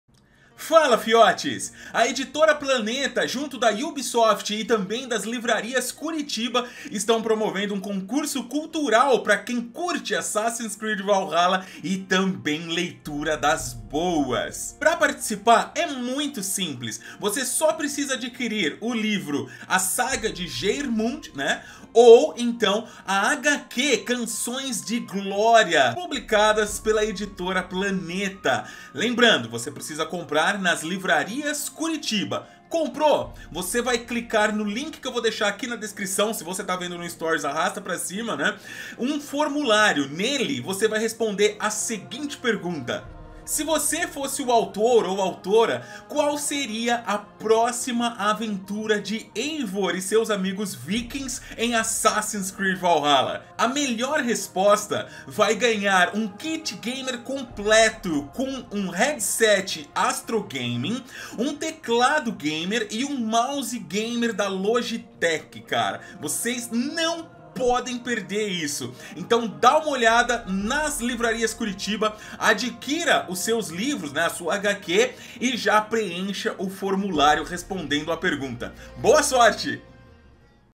GANHE UM KIT GAMER COMPLETO da @Ubisoftbrasil, @GrupoLivrariasCuritiba e
GANHE UM KIT GAMER COMPLETO da @Ubisoft Brasil , @Grupo Livrarias Curitiba e

• Conheça as Regras:
- Para participar do concurso cultural é necessário comprar, no mínimo, 01 exemplar de Assassin's Creed© Valhalla: A saga de Geirmund e 01 exemplar de Assassin's Creed Valhalla: Canção de Glória, na Livrarias Curitiba. É necessária a compra dos dois títulos, pelo menos um exemplar de cada;
- Para concorrer aos prêmios, é necessário preencher os dados pessoais e responder à pergunta no formulário de participação (somente a compra dos livros não garante a participação no concurso cultural);
- Também é necessário encaminhar, no mesmo formulário, uma imagem do comprovante de compra dos livros na Livrarias Curitiba;
- O concurso não tem validade para quem comprou outros livros da saga Assassin's Creed;
- O concurso não tem validade para quem comprou os livros em outra livraria;
- O período de inscrições é de 05 a 31 de julho de 2021. Não serão considerados formulários preenchidos após essa data;
- O vencedor precisa residir em território nacional.

🔻🔻 SEJA MEMBRO 🔻🔻

🔻🔻 CONECTE-SE COMIGO 🔻🔻

Kallil Augusto